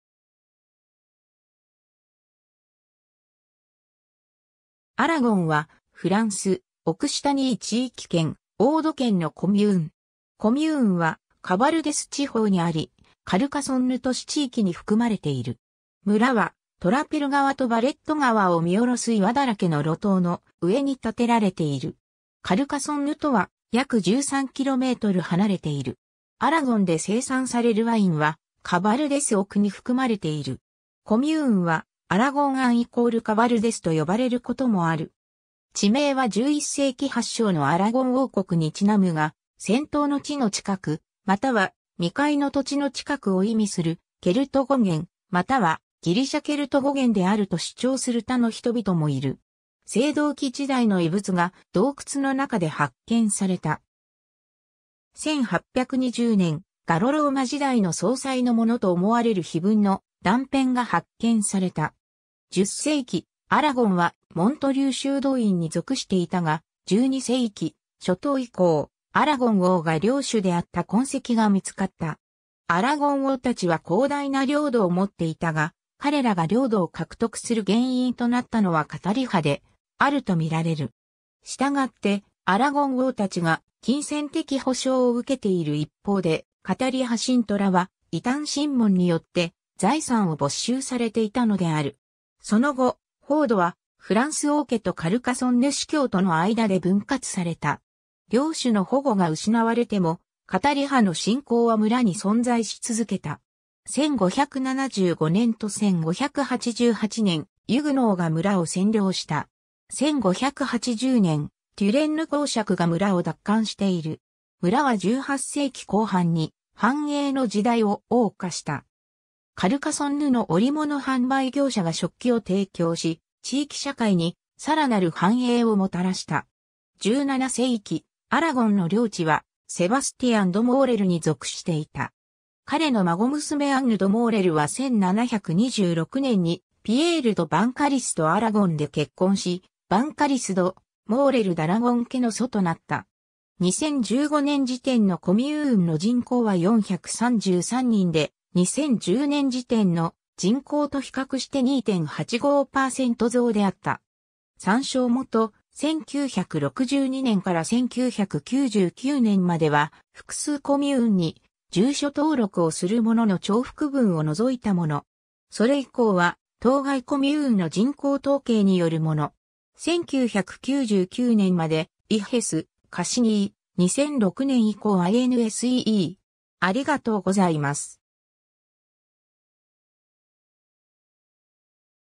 アラゴン (オード県)
アラゴン （Aragon、オック語:Argon）は、フランス、オクシタニー地域圏、オード県のコミューン。
 コミューンはカバルデス地方にあり、カルカソンヌ都市地域に含まれている。
村は、トラペル川とヴァレット川を見下ろす岩だらけの露頭の上に建てられている。
カルカソンヌとは約13km離れている。
 アラゴンで生産されるワインは、カバルデスAOCに含まれている。
 コミューンはアラゴン＝アン＝カバルデス（Aragon-en-Cabardès）と呼ばれることもある。
 地名は11世紀発祥のアラゴン王国にちなむが、『戦闘の地の近く』または『未開の土地の近く』を意味するケルト語源またはギリシャ＝ケルト語源であると主張する他の人々もいる。
 青銅器時代（紀元前2000年頃）の遺物が、洞窟の中で発見された。
1820年、ガロ＝ローマ時代の葬祭のものと思われる碑文の断片が発見された。
 10世紀、アラゴンはモントリュー修道院に属していたが、12世紀初頭以降、アラゴン王が領主であった痕跡が見つかった。
アラゴン王たちは広大な領土を持っていたが、彼らが領土を獲得する原因となったのはカタリ派であるとみられる。
したがって、アラゴン王たちが金銭的補償を受けている一方で、カタリ派信徒らは異端審問によって財産を没収されていたのである。
その後、封土はフランス王家とカルカソンヌ司教との間で分割された。
領主の保護が失われても、カタリ派の信仰は村に存在し続けた。
 1575年と1588年、ユグノーが村を占領した。
1580年、テュレンヌ公爵が村を奪還している。
 村は18世紀後半に繁栄の時代を謳歌した。
カルカソンヌの織物販売業者が織機を提供し、地域社会にさらなる繁栄をもたらした。
 17世紀、アラゴンの領地はセバスティアン・ド・モーレルに属していた。
彼の孫娘アンヌ・ド・モーレルは1726年にピエール・ド・バンカリスとアラゴンで結婚し、バンカリス・ド・モーレル・ダラゴン家の祖となった。
 2015年時点のコミューンの人口は433人で、2010年時点の人口と比較して2.85%増であった。
1999年までEHESS/Cassini、2006年以降INSEE。